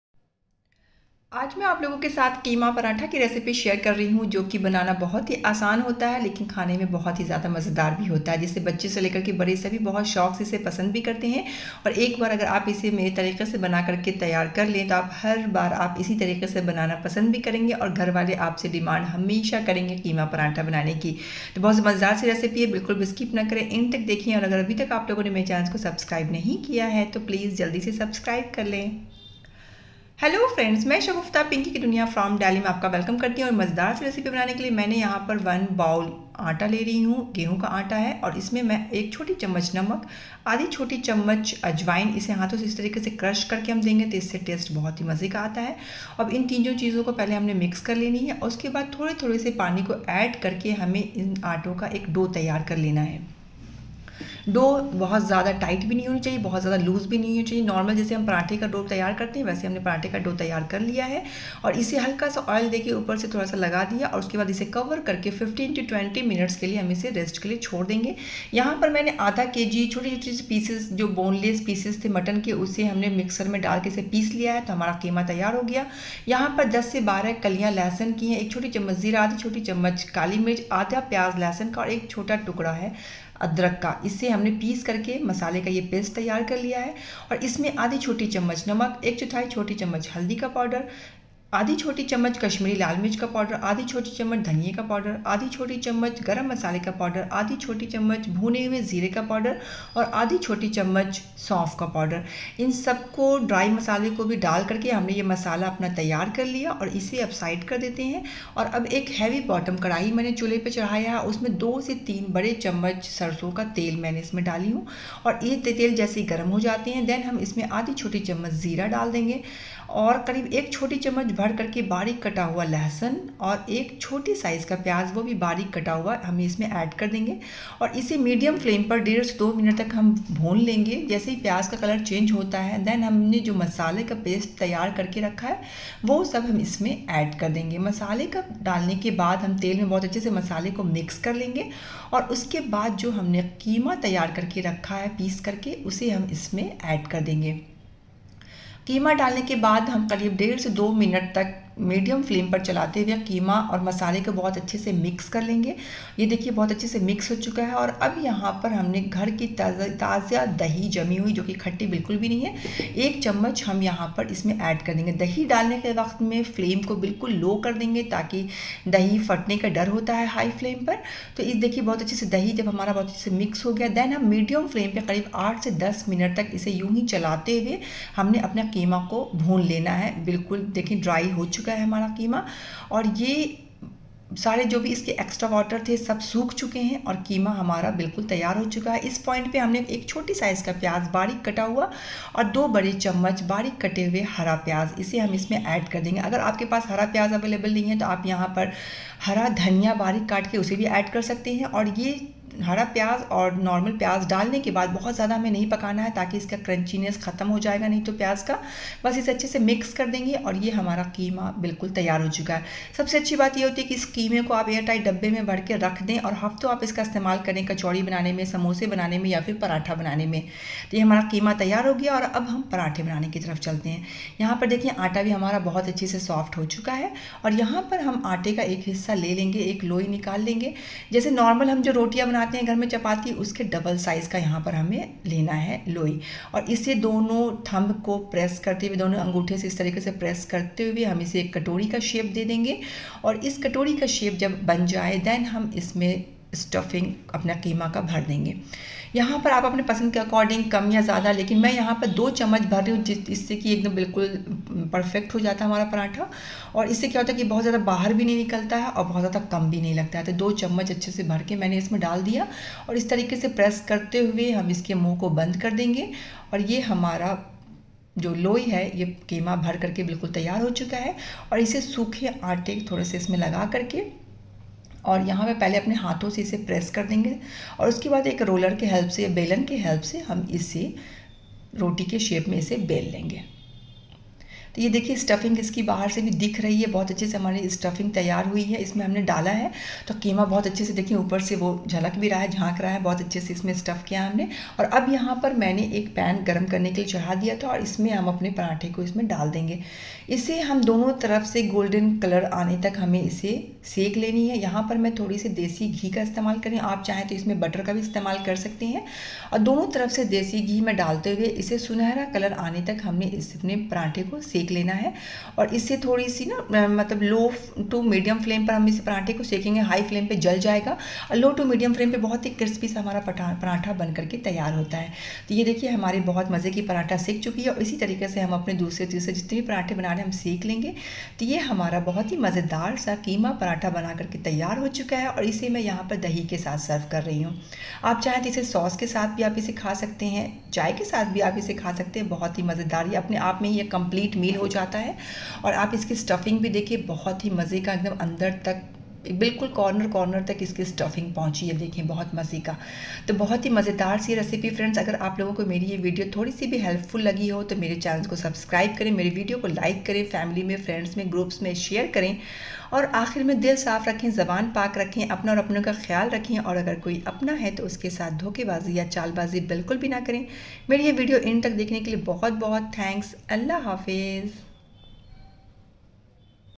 keema Paratha Recipe/मटन कीमा पराठा/How To Make Keema Paratha At Home🏠 Pinky Ki Duniya From Delhi 💞
Keema Paratha Recipe/Paratha Recipe/Mutton Keema/How To Make Keema Paratha At home 🏠 Pinky Ki Duniya From Delhi 💞

Friends, if you like my video then
 help to grow:

Like 👍this video and leave comment♥️